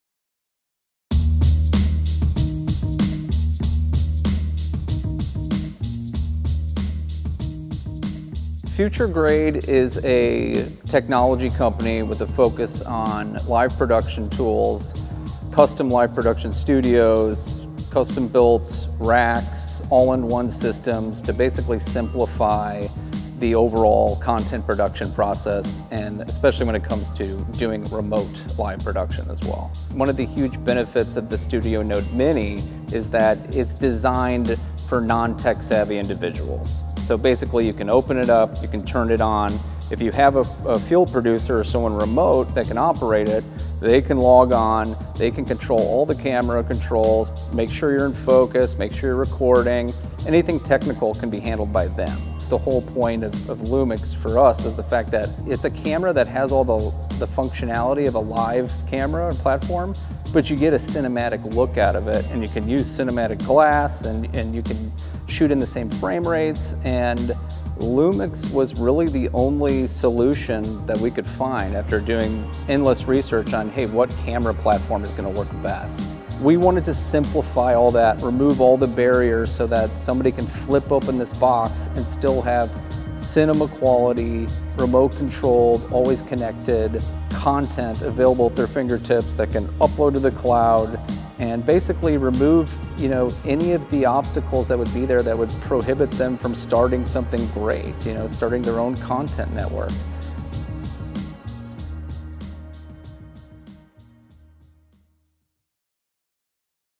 BS1H: Future Grade’s Studio Node Mini
With the LUMIX BS1H, Future Grade’s Studio Node Mini merges broadcast and cinema in a compact remote package that is ideal for content creation or live production.